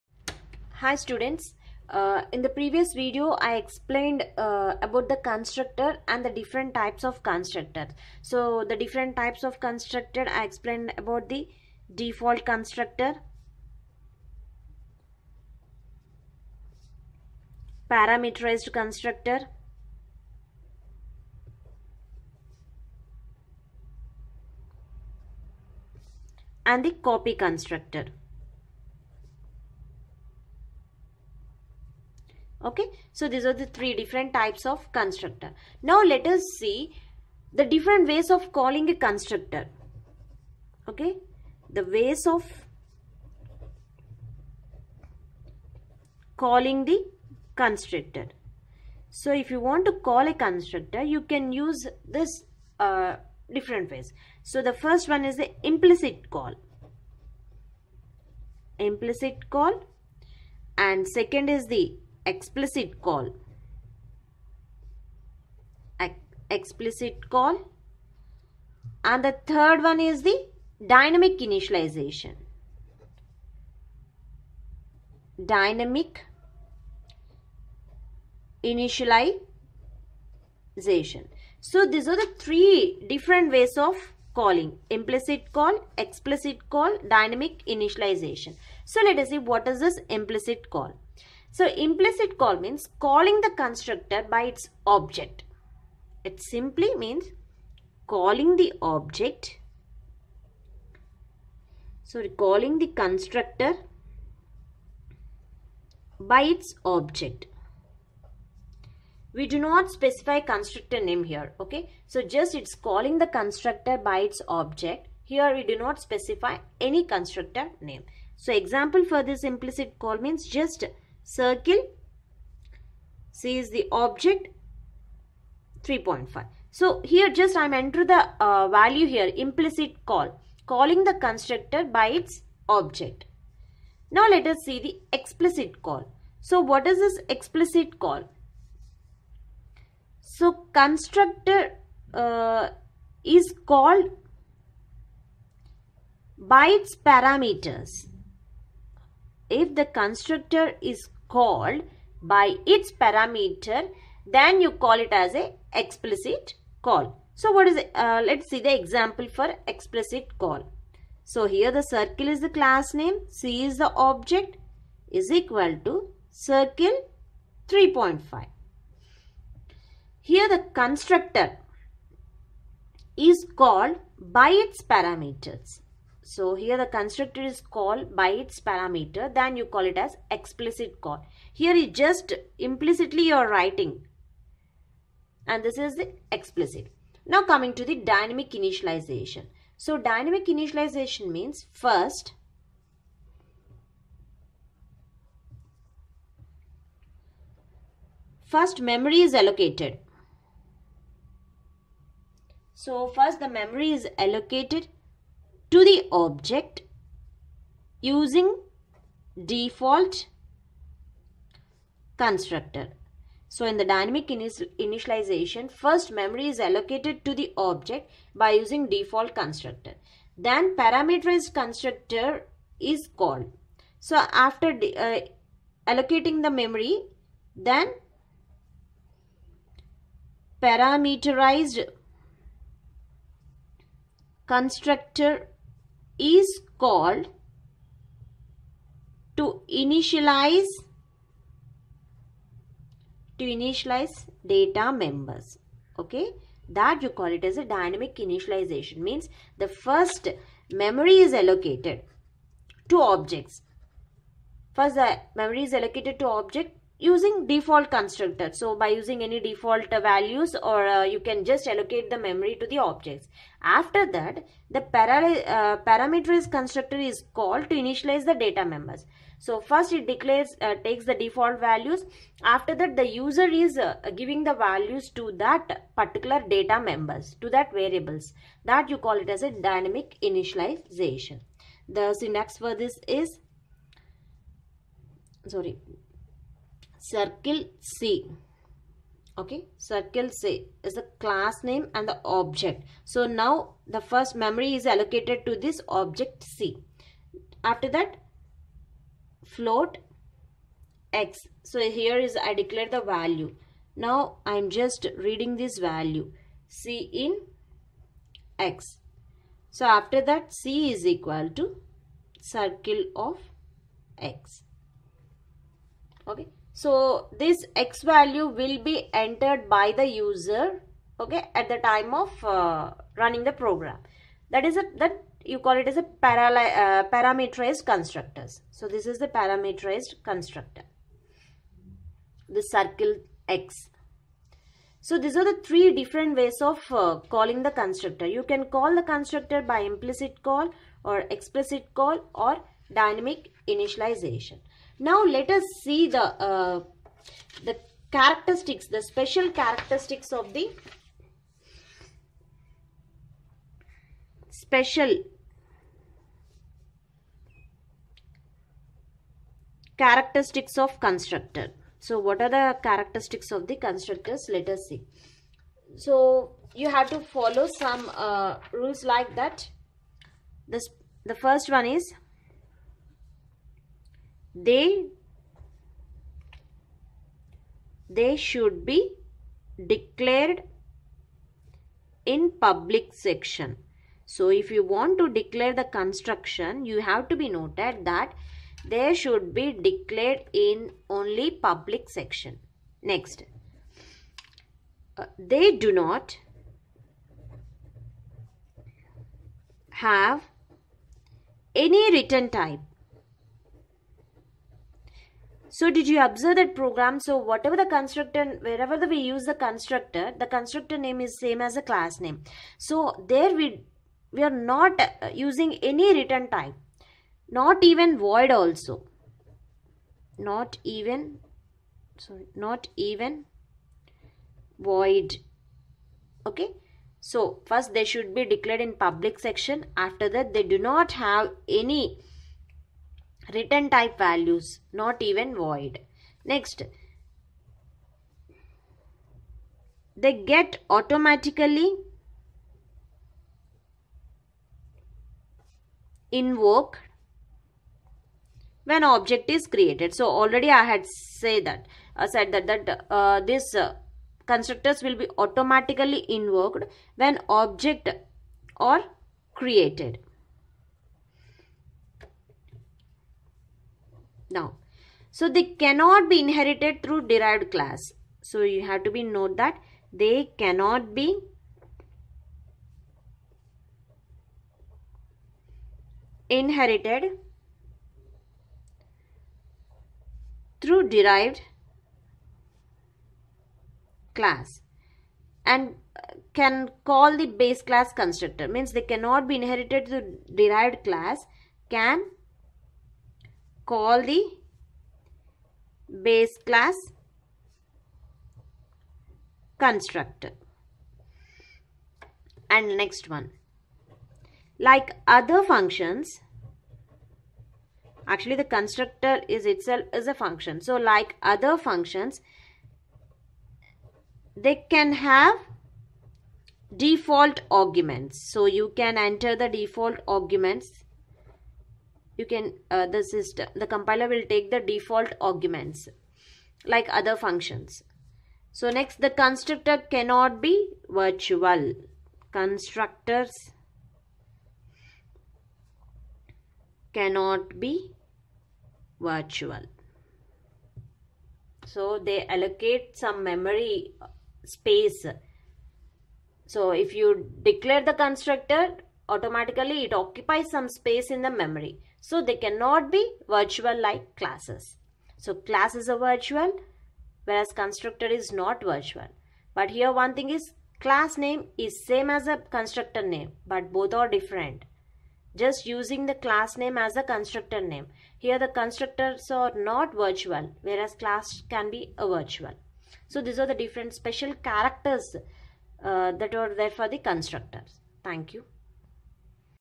Constructor in C++ | Calling types | Part-2/2 | OOPs in C++ | Lec-18 | Bhanu Priya
Object-Oriented Programming ( OOPs )
Constructor in C++:
Implicit call,Explicit call, Dynamic Initialization & Special Characteristics explained